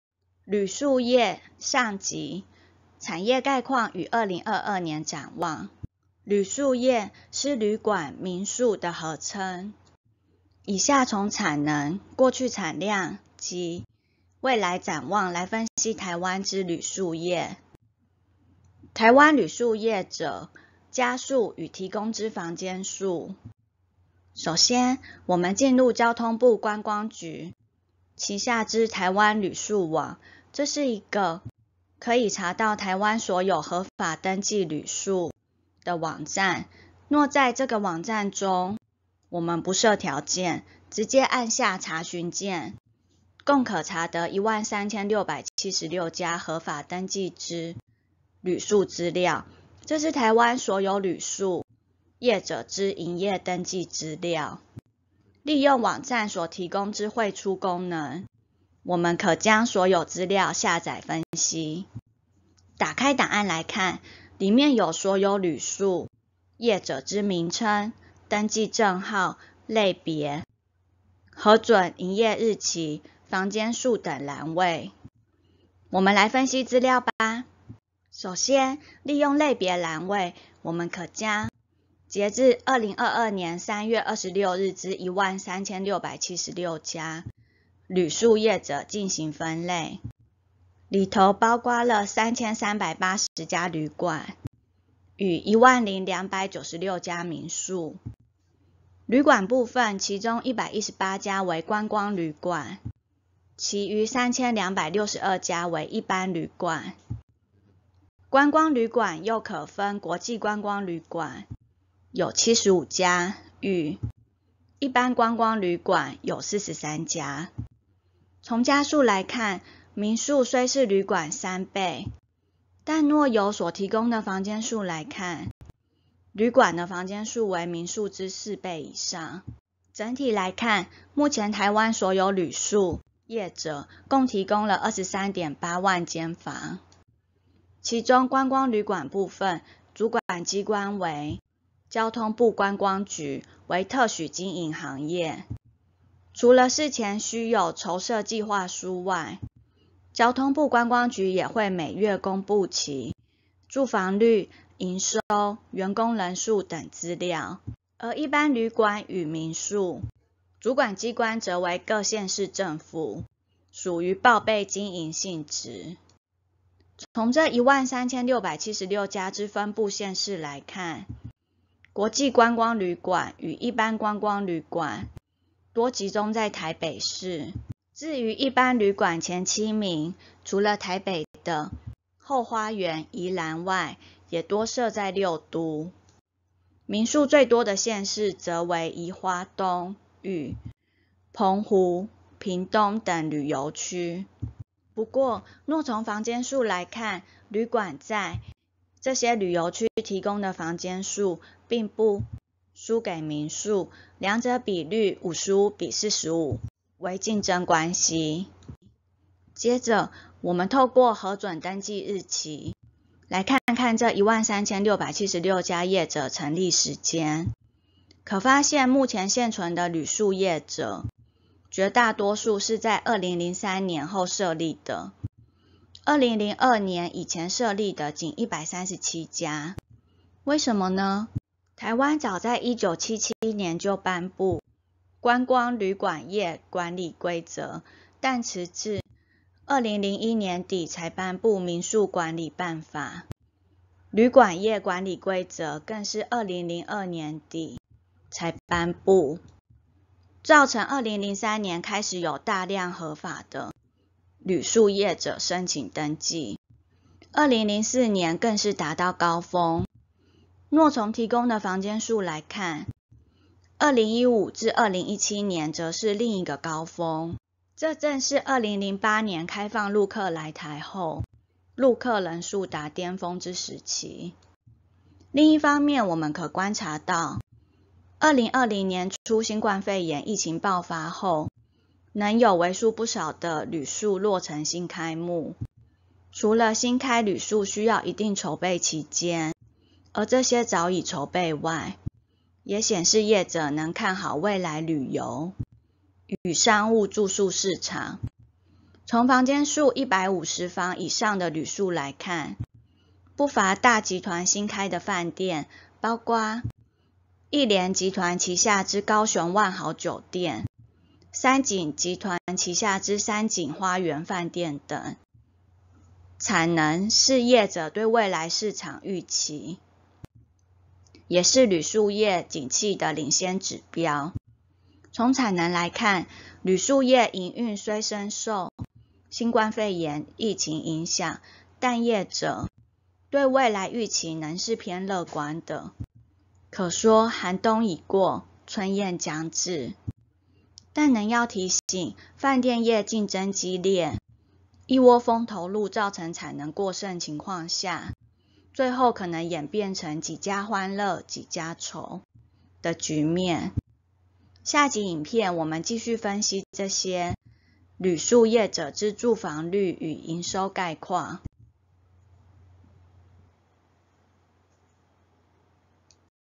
【旅宿業】(上集) -產業概況與2022年展望
旅宿業是旅館業（飯店業）、與民宿業合稱。近3年新開幕之旅館有哪些、新增多少房間？台灣所有旅宿業者共幾家、分佈縣市為何？本影片帶您一塊來瞭解。